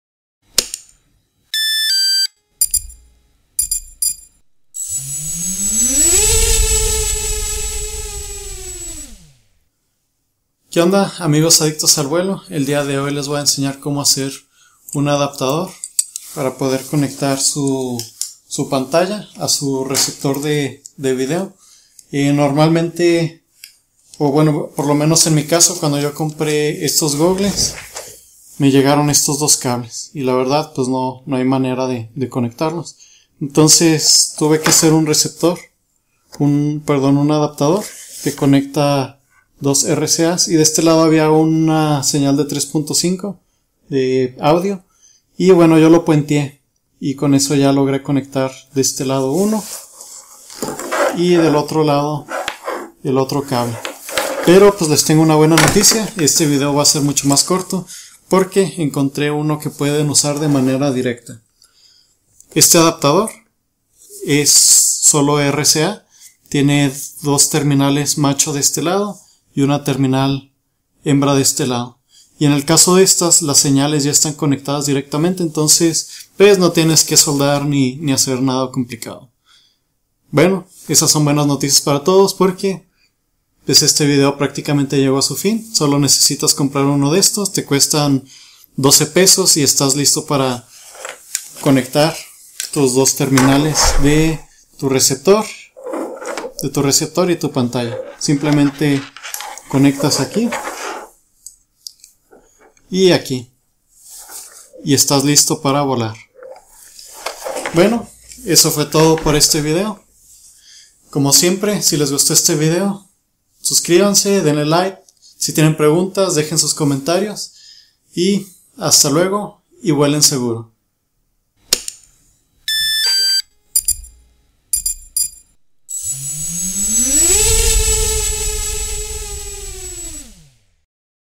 Adaptador para conectar 2 Plugs RCA
En este video podrás ver como conectar 2 plugs RCA.

Solo como cultura general de conectores:
Conector Hembra (Jack/Receptacle) - Terminal macho
Conector Macho (Plug) - Terminal hembra
Es raro que un conector hembra tenga terminales hembra. Pero quiero mencionarlo aquí por que esto causa bastante confusión a las personas que están entrando al mundo de los arneses.